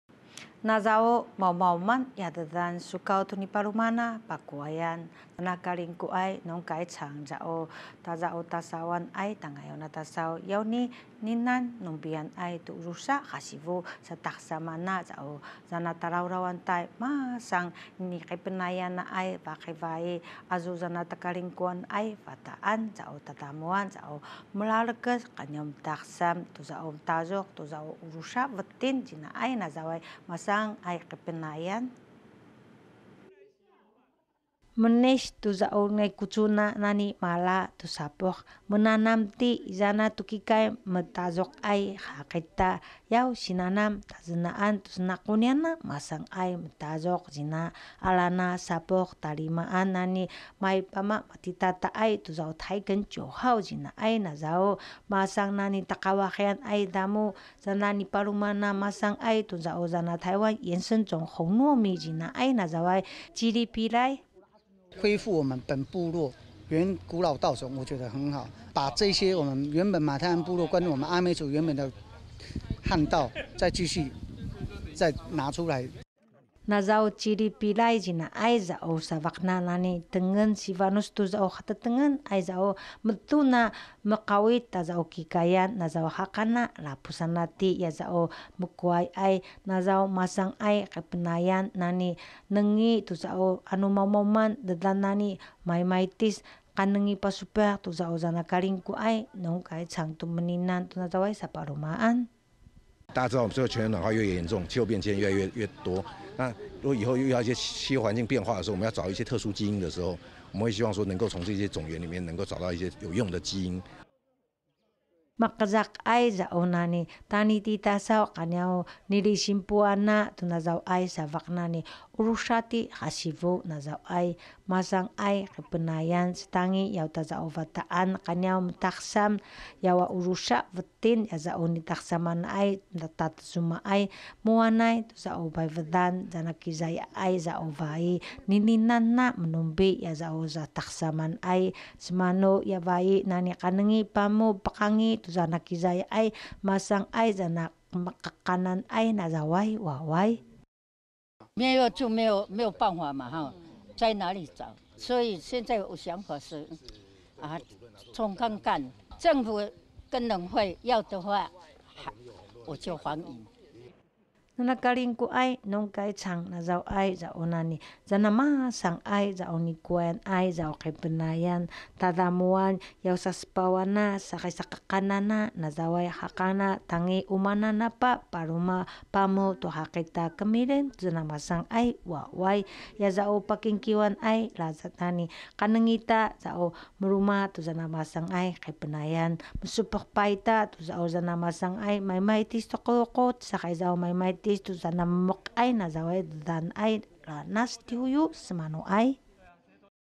保存台灣原生種稻作 花蓮馬太鞍復育 2019-04-02 Kavalan IPCF-TITV 原文會 原視族語新聞
脫下鞋子拿起翠綠的秧苗，使用插秧機習慣的農民，正在田裡學習如何傳統插秧，他們手上的秧苗並不是常見的台梗九號，而是早期東部地區原住民常種的台灣原生種紅糯米"吉利比賴"。

"吉利比賴"因稻穗部分有黑色硬毛，會導致打鼓機卡住因此被放棄種植，但這種原生種稻作也因古老，可能有抗極端氣候的基因，所以引起花蓮農改場重視。

事實上農改上這幾年已蒐集200多種台灣原生種稻作，這次在光復鄉種植的品種共有20種，很感謝在地一位80歲的阿美族阿嬤提供珍藏的種子，阿嬤則是希望可以幫忙阿美族恢復傳統米食文化。

花蓮農改場強調，這些原生種稻穀有很多都是部落傳統的祭祀食物，所以保種復育也可以幫忙原住民找尋失傳的文化，研究人員則是會從這些原生稻米上，找尋抗逆境、病蟲害、耐旱等基因。